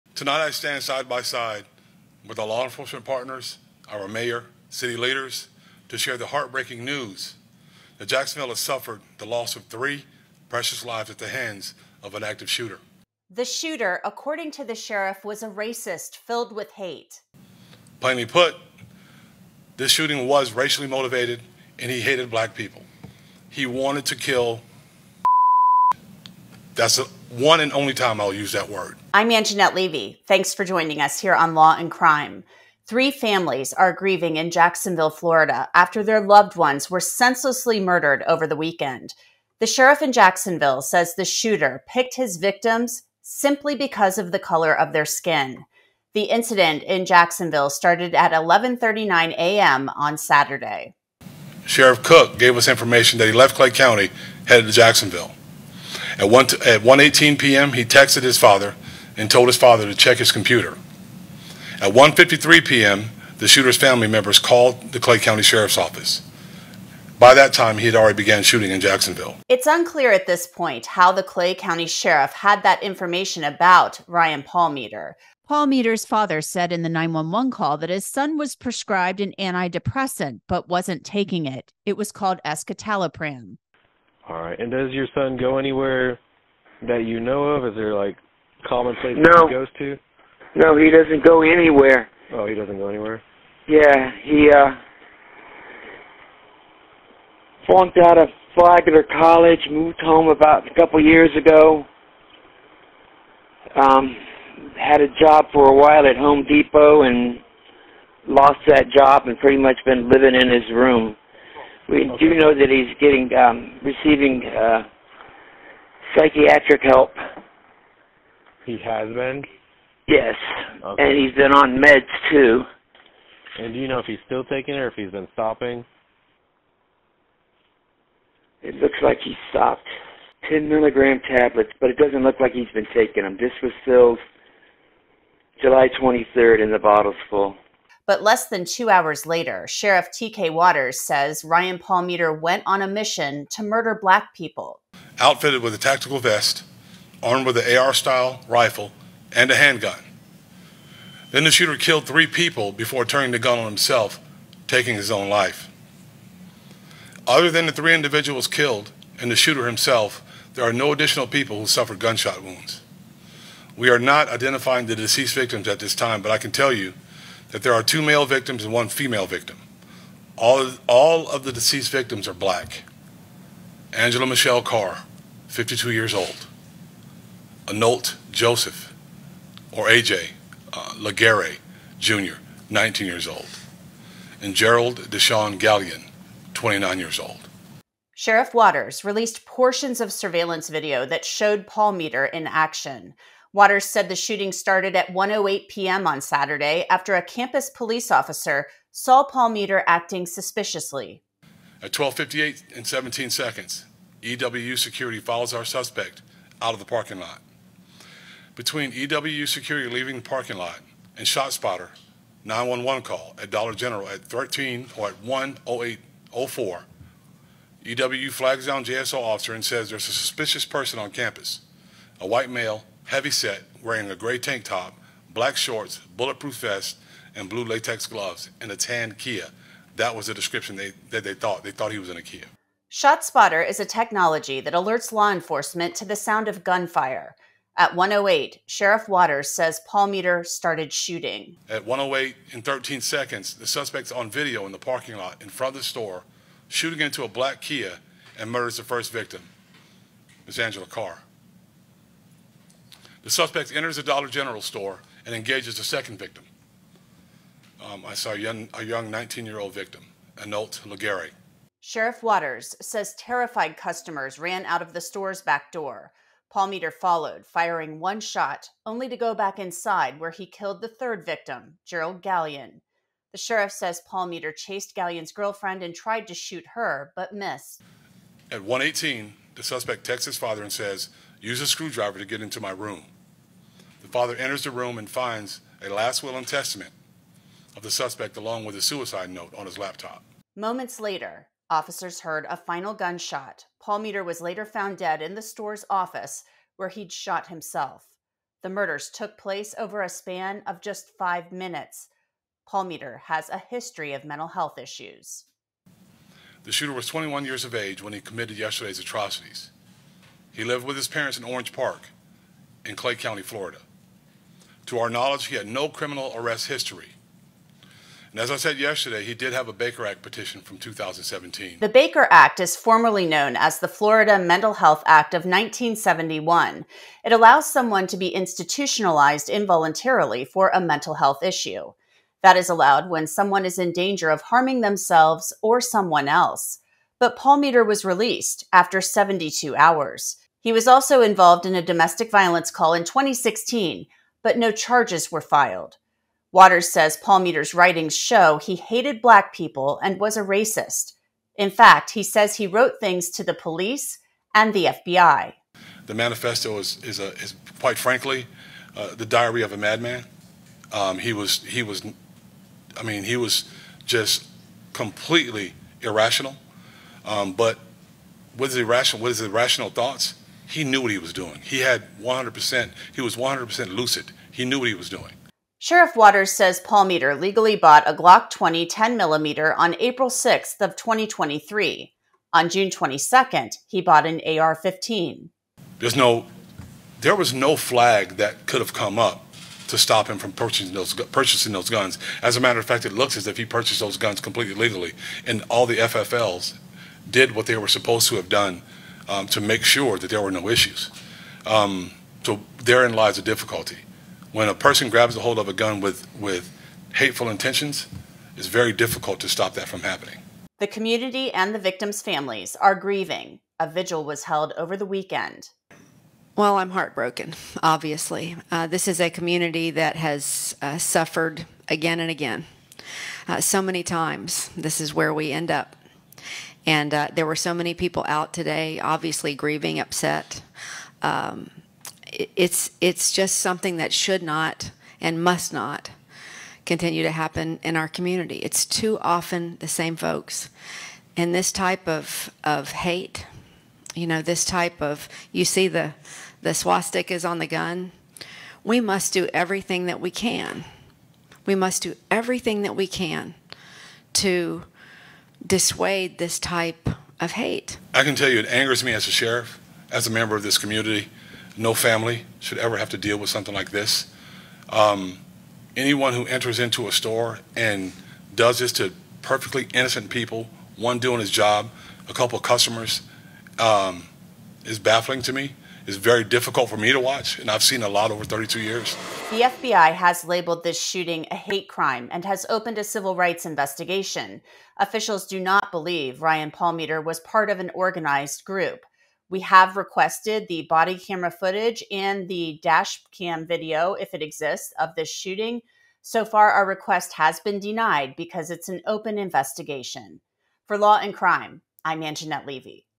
Florida Man Murders 3 People in Racially-Motivated Killing Spree — Everything We Know
A racially-motivated killing spree sent shock through the city of Jacksonville, Florida after a gunman armed with a swastika-emblazoned assault rifle opened fire at a Dollar General on August 26. The Law&Crime Network’s Angenette Levy details everything we know so far.